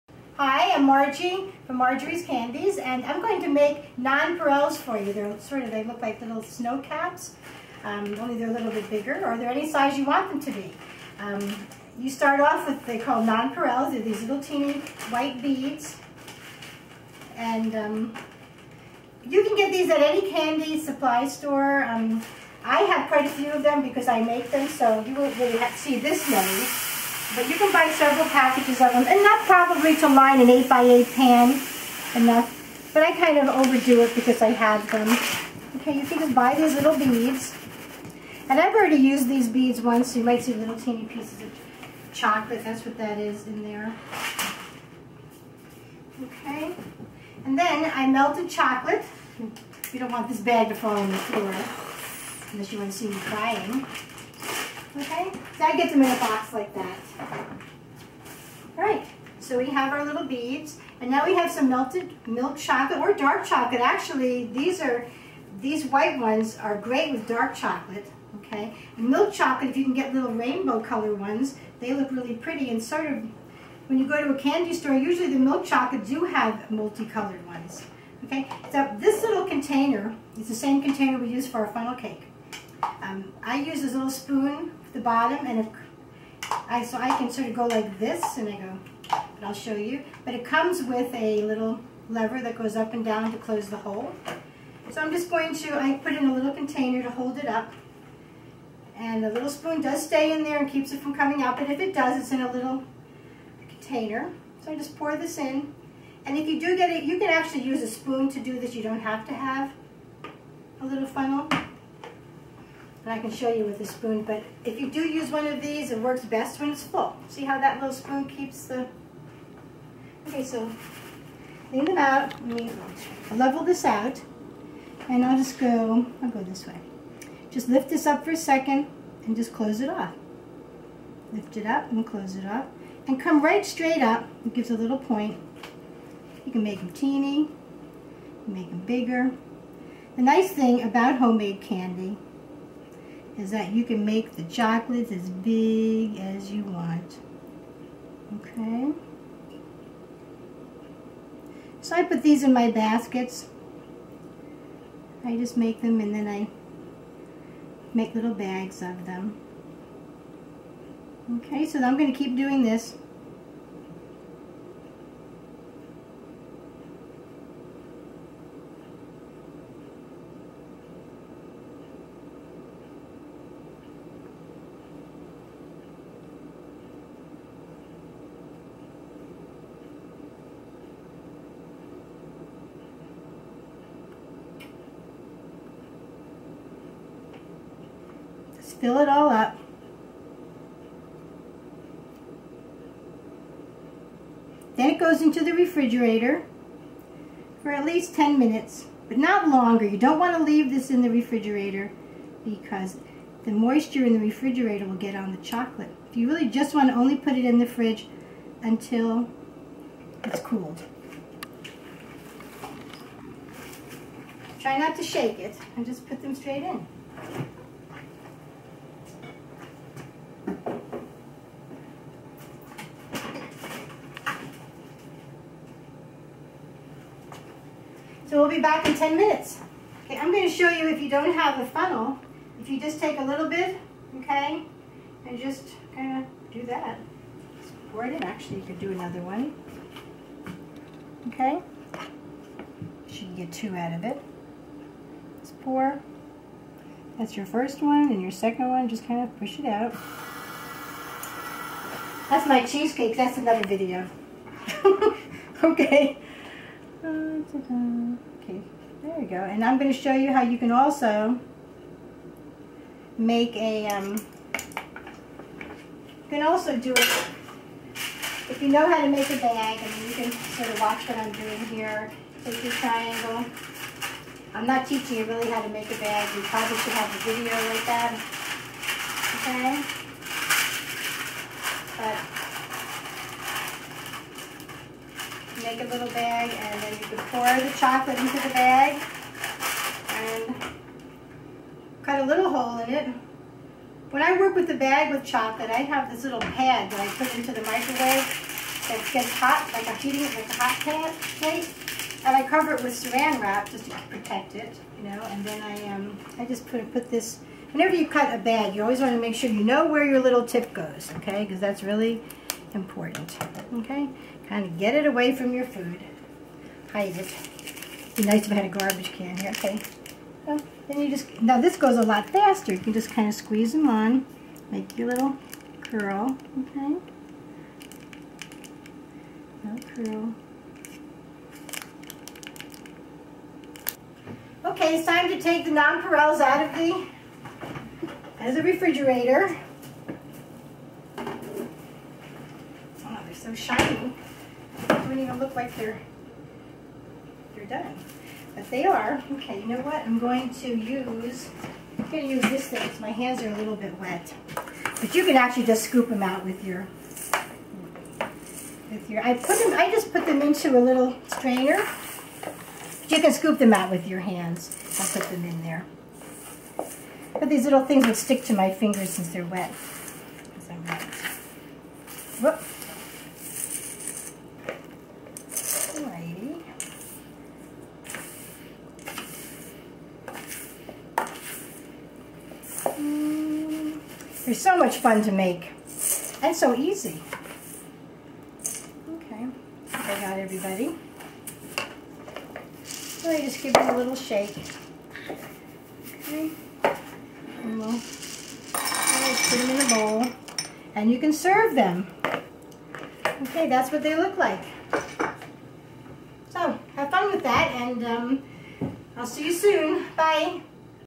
Marjorie's Candies Chocolate Snow Caps or Nonpareils
Snow Caps or Nonpariels
Chocolate, melted NOTE BELOW
Nonpariel candy beads
Funnel and a wooden spoon which has a long handle that has a rounded tip at the end of the handle (that will close off the whole at the end of the funnel)
1.  Place the nonpariel candy beads in a plastic lined tray and shake to level them out.
2. Place the funnel in a container to hold it so it doesn't leak when you add the chocolate to it.
3.  Put the handle tip down into the funnel to close off the whole and pour the chocolate into the funnel. My funnel works the best when it is very full. It helps the chocolate come out of the funnel better and faster.
4  Start the make the nonpariels or snow caps by placing the funnel over the nonpariel beads and very quickly lifting and closing back off the spoon to allow the chocolate te fall out of the funnel. Allow only about 1 tsp to fall from the funnel. (actually the nonpariels may be as large or as small as you want them to be). Continue to do this until the tray is ful.
5.  Place the tray in the refrigerator for about 10 minutes or until the candies have completely hardened. . Try not do shake the tray while the chocolate is melted and do not leave chocolate in the refrigerator longer because the refrigerator has too much moisture in it and that will settle on the surface of the chocolate.
6.  Remove the tray from the frig and gentle scoop the chocolates out of the candy beads allowing all the bead to fall away form the chocolates.
7  Store in a sealed container.
 NOTE:  When the chocolate is melted  a few drops of any oil may be added for flavor OR if using white chocolate also different colors may be added for favor themes.  Just be sure to use colors and flavors which are designed for adding to chocolate.